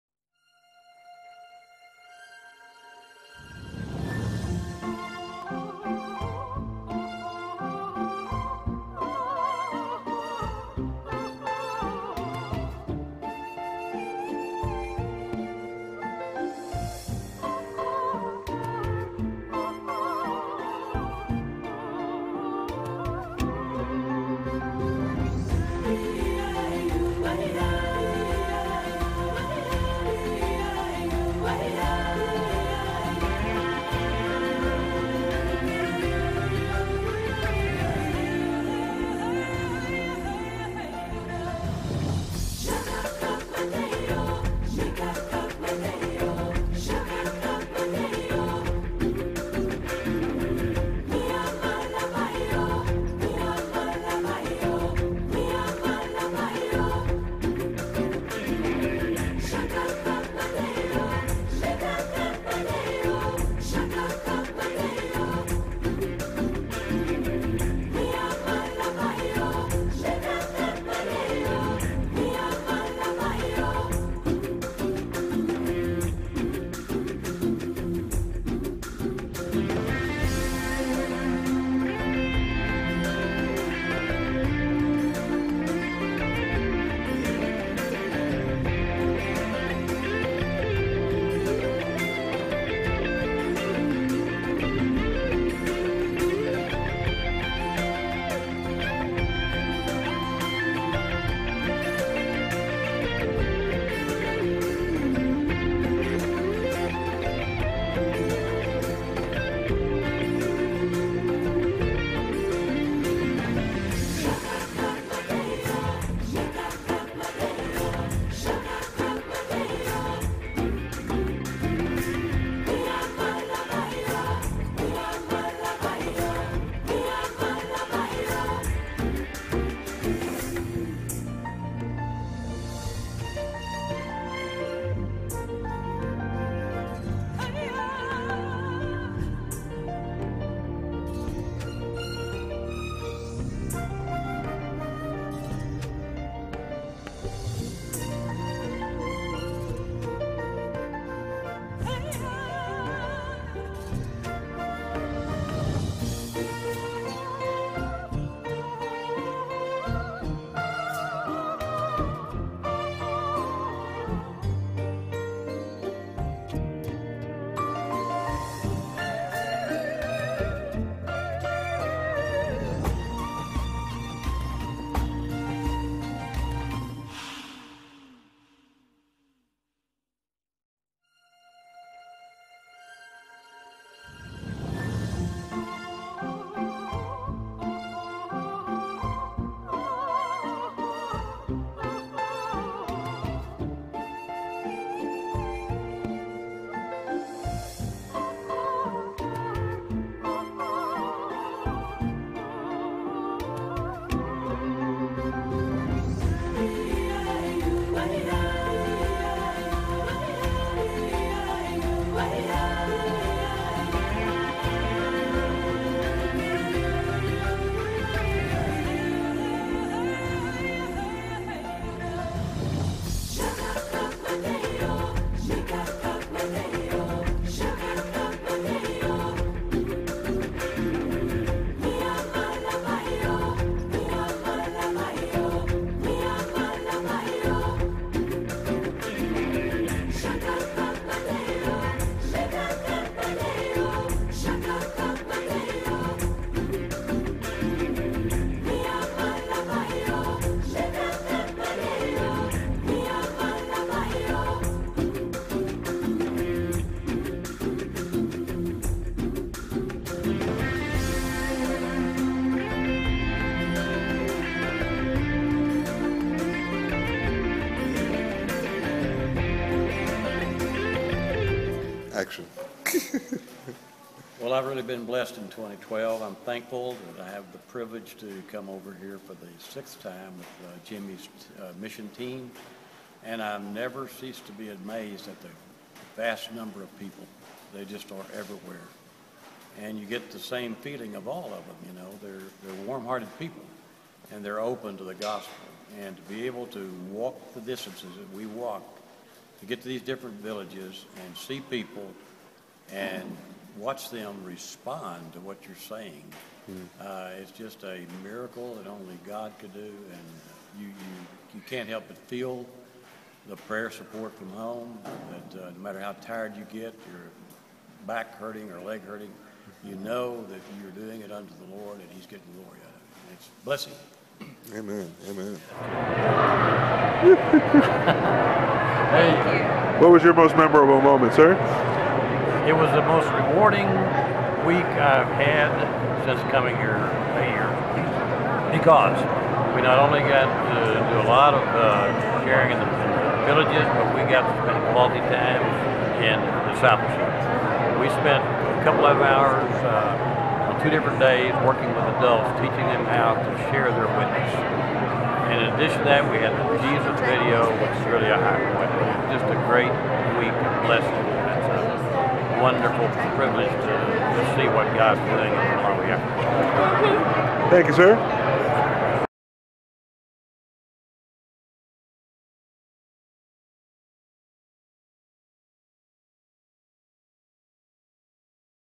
2021 May - Clyde Kemp Malawi Memories
Memorial slideshow of various images from the Malawi mission field of Bro Clyde Kemp.